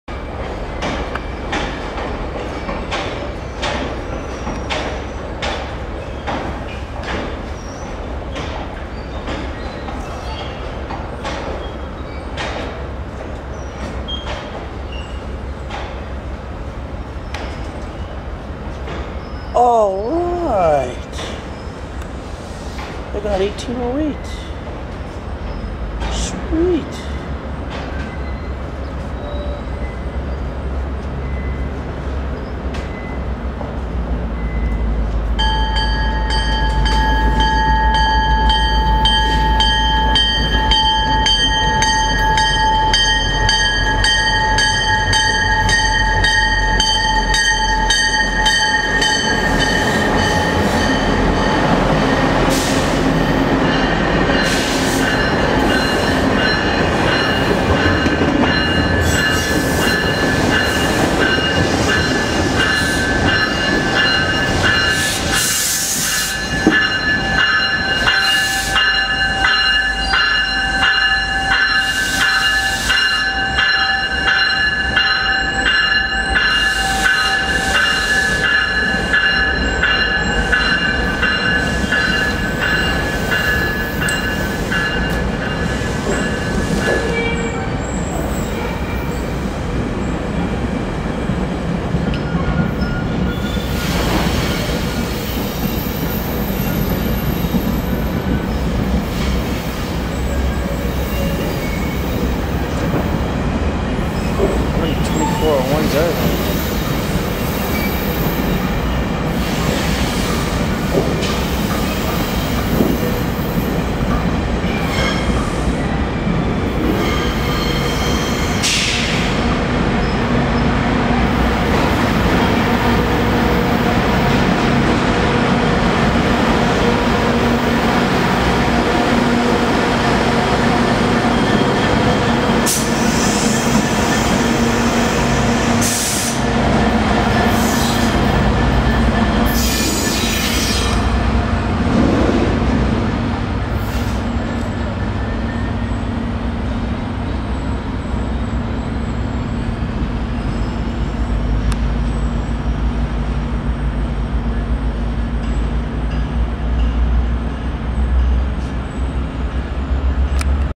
ONR Northlander and GO Train
this is Northlander 697 with 1808, 202, 614, 606 & 700 on February 22nd 2010 coming into track 1 at Toronto Union Station, 697 left 25 minutes late due to GO Trains coming and going on Track 1 - the track 697 was on !
I seen 697 stopped beside the CN Tower waiting to get the clearance to come in !
as much as I love the Northlander and hate to say this - the Northlander doesn't even come close to being high on the priority list for Union Station departures !    The Union Station Rail Corridor controller isn't going to hold up a 12 car GO Train which can transport up to 2400 people or a 10 car train that can move up to 2000, about 200 people per car for a train that only carries up to 30 people at any one time to go nowhere !
I'm NOT saying that to offend anyone - I'm just speaking the TRUTH !
NEGATIVE COMMENTS WILL BE DELETED AND THOSE WHO POST THEM WILL BE BLOCKED ! ! ! ! !